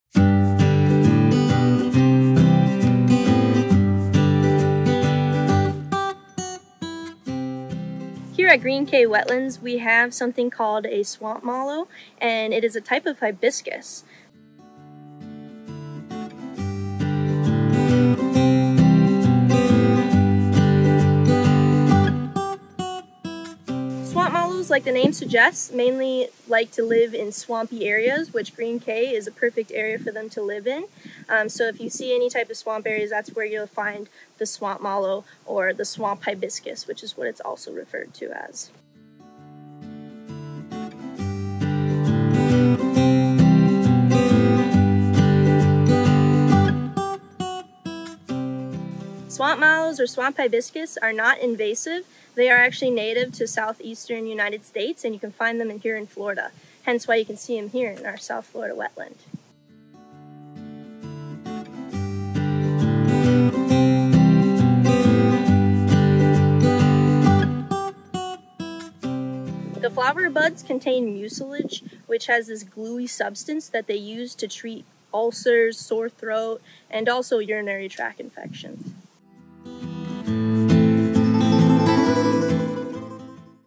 What's that Plant? Swamp Mallow!
Swamp Mallow also referred to as Swamp Hibiscus are commonly found in swamps much like south Florida wetland areas. Watch this episode of "What's That Plant" to find out what this healing hibiscus is also used for.